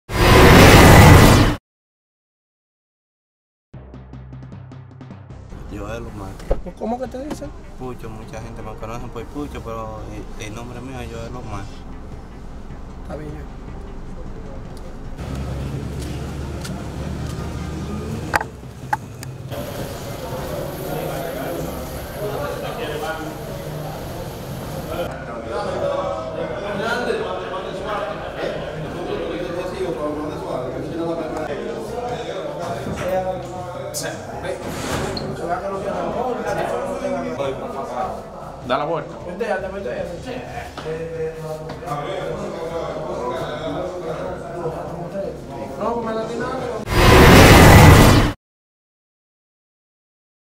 Se entrega joven acusado de varios hechos delictivos en SFM
Se entregó el jueves a las autoridades policiales, un joven que era buscado por supuestamente cometer varios hechos delictivos en esta ciudad.

Yoel Medina, de unos 23 años de edad, está siendo señalado como el responsable de despojar a varias personas de sus motocicletas en medio de asaltos en esta localidad.

Medina dijo que en reiteradas ocasiones miembros de la Policía Nacional habían irrumpidos su vivienda.

Por el momento las autoridades no han dado mayores detalles.